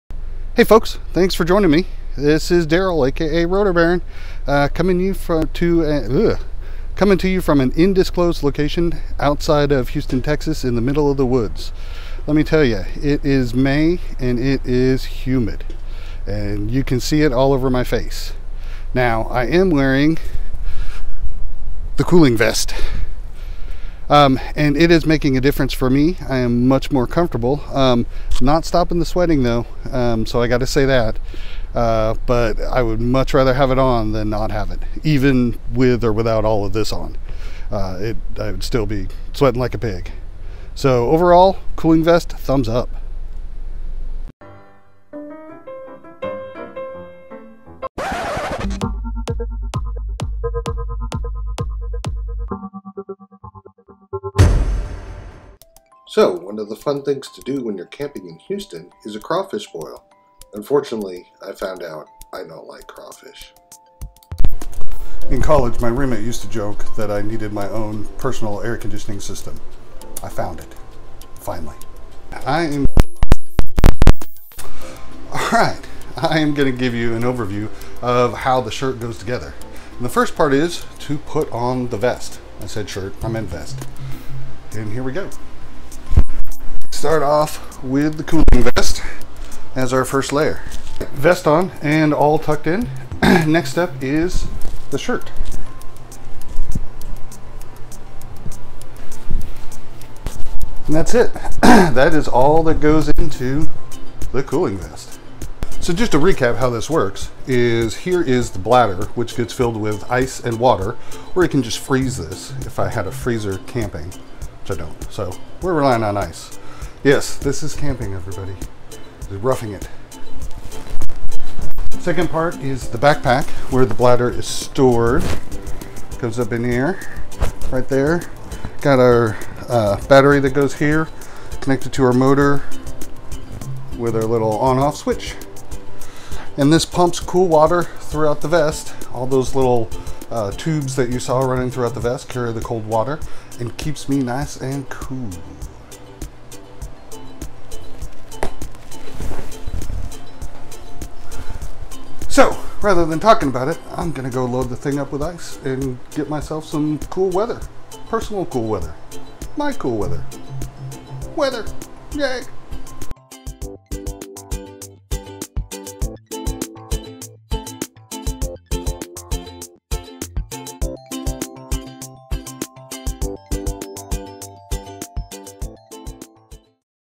A Cool Update
A quick update on how my cooling vest kept me in comfort in May, near Houston, Texas.

Watch this latest update to my pursuit to be cool in a warm world.